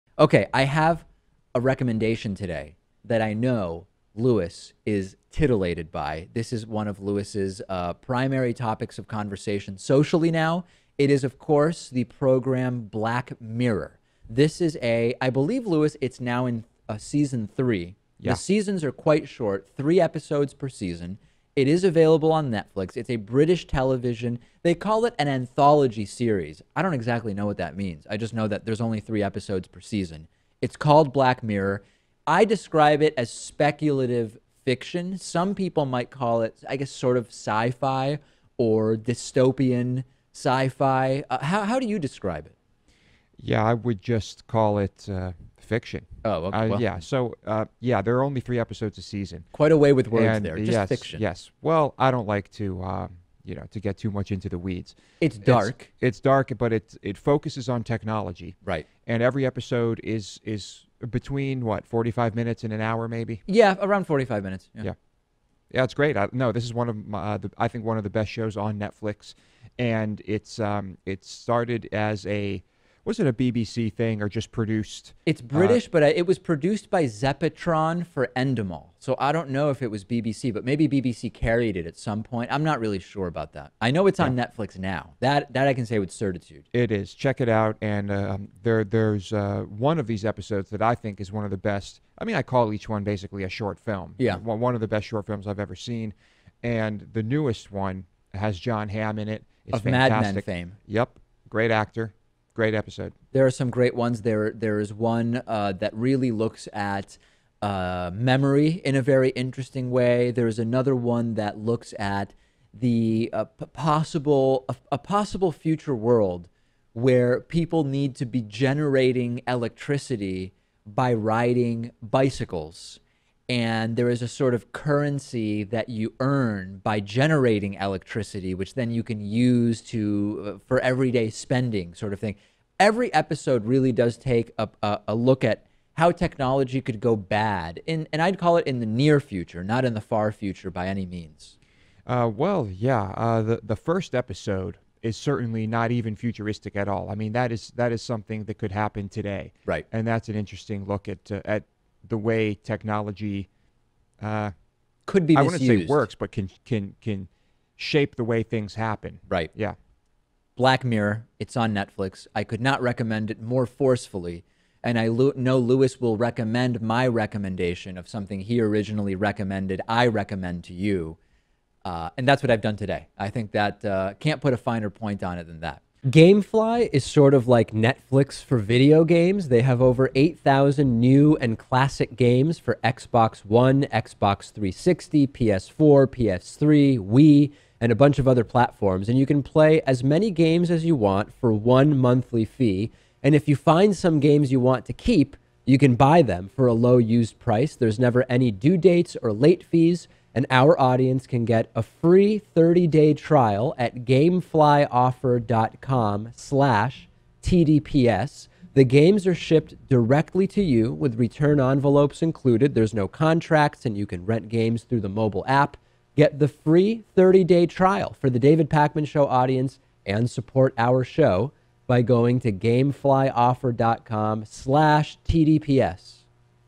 Recommendation: Black Mirror
--Show recommendation: Black Mirror

On the Bonus Show: The NFL warns its players about meat from Mexico and China, homeless Italians reject vegan meals, a tow truck driver refuses to help a Bernie supporter, more...

24/7 Voicemail Line: (219)-2DAVIDP